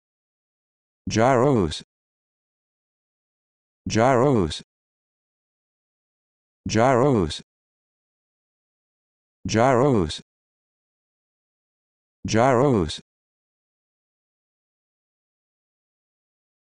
How to Pronounce Gyrose
This video shows you how to pronounce Gyrose